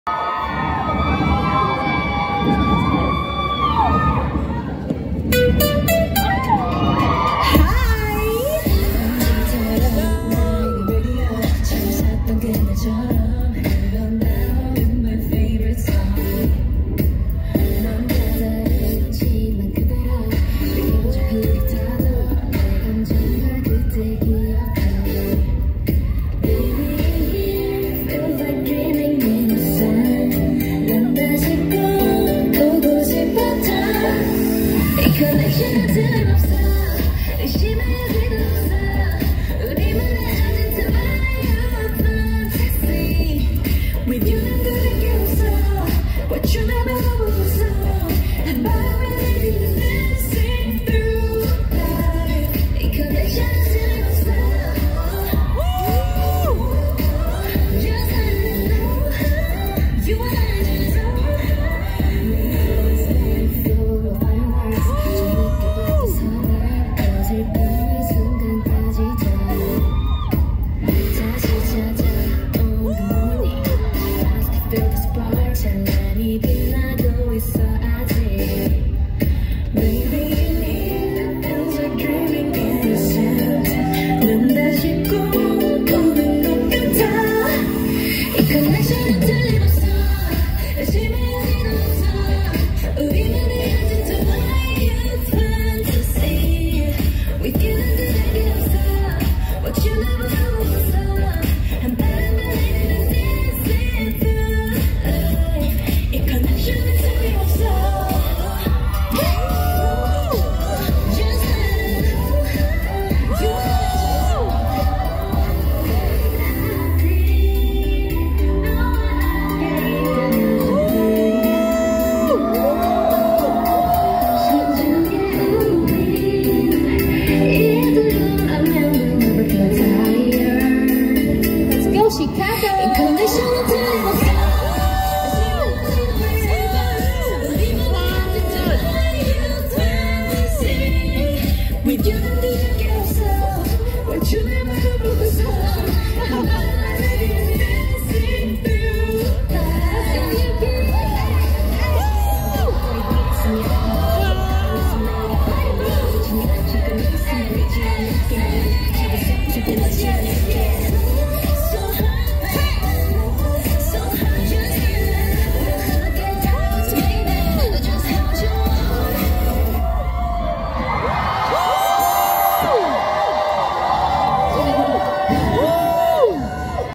[4K] PURPLE K!SS (퍼플키스) in Chicago [AGIT]
Recording at Center Theater at North Shore Center 20231116.

This video is solely for the purpose of sharing the experience and spreading the word about PURPLE KISS (퍼플키스), all songs are owned by RBW Entertainment.